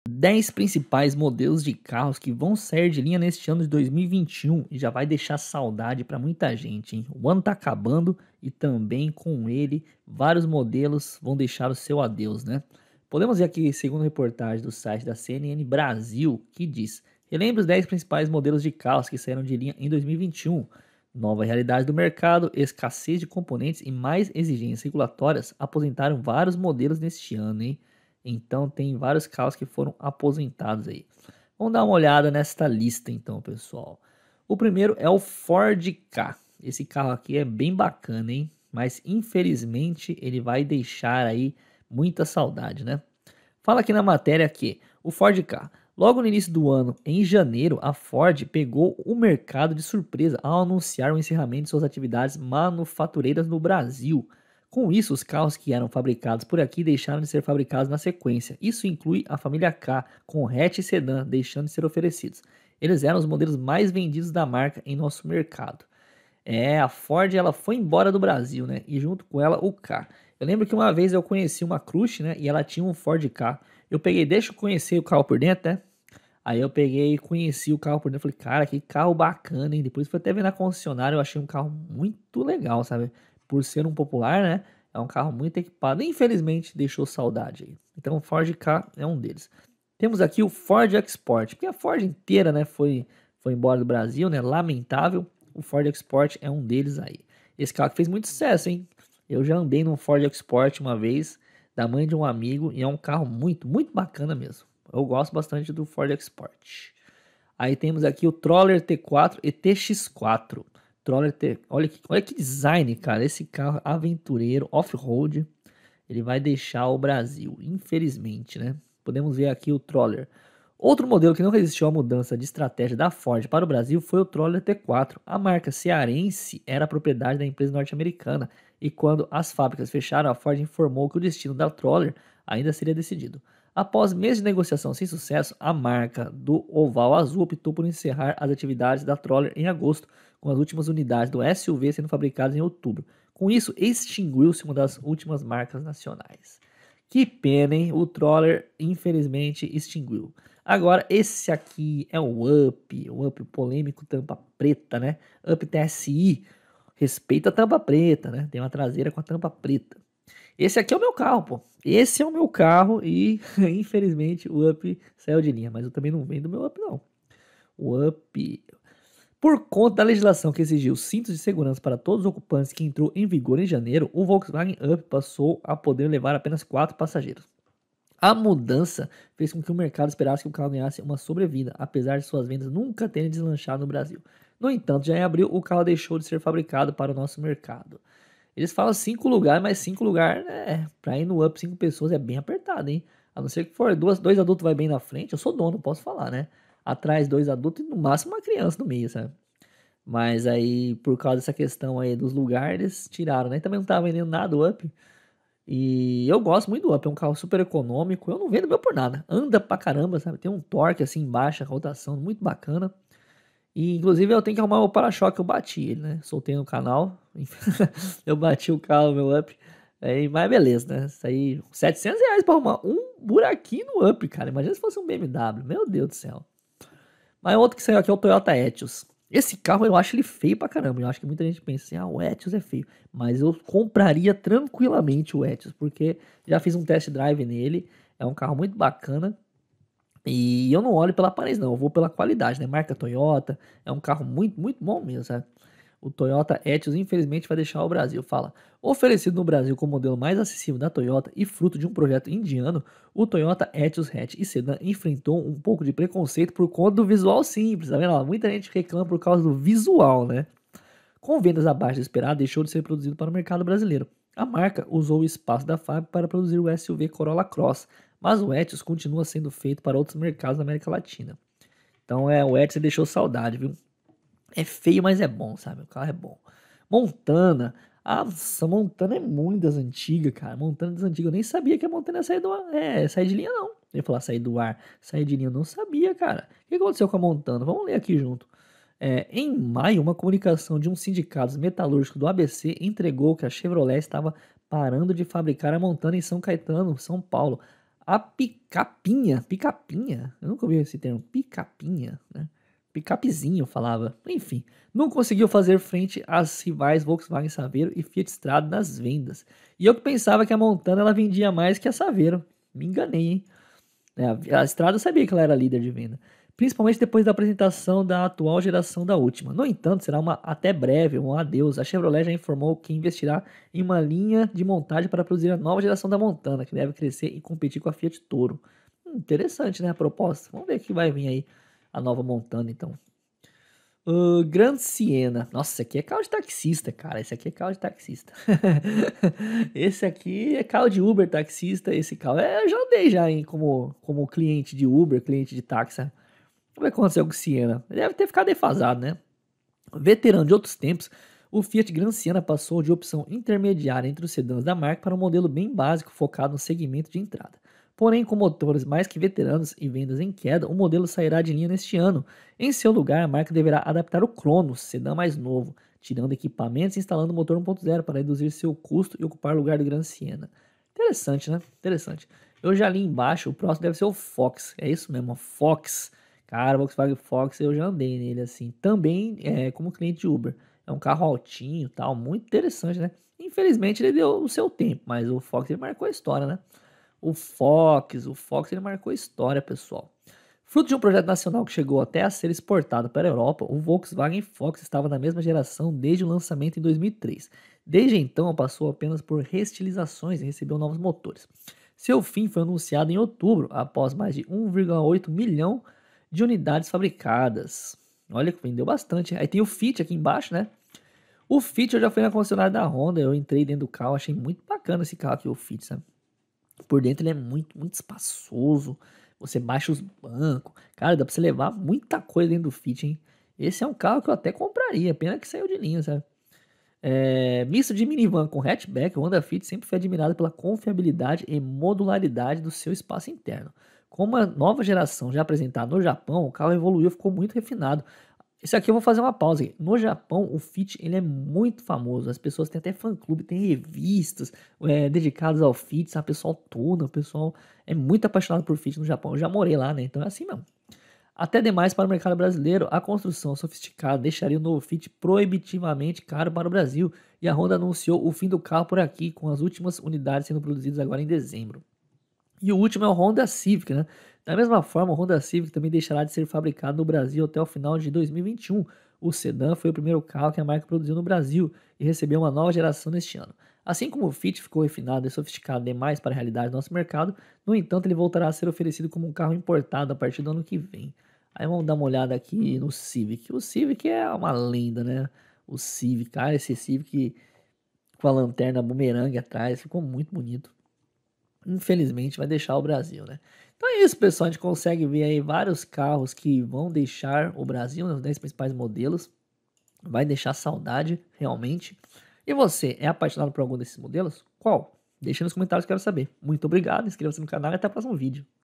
ADEUS 2021! DEZ MODELOS DE CARROS SAIRAM DE LINHA NO BRASIL !
Relembre os 10 principais modelos de carros que saíram de linha em 2021
Nova realidade do mercado, escassez de componentes e mais exigências regulatórias aposentaram vários modelos neste ano

Ford Ka
Ford EcoSport
Troller T4 e TX4
Volkswagen up!
Toyota Etios
Chevrolet Montana
Fiat Grand Siena
Volkswagen Fox
FIT
CIVIC HONDA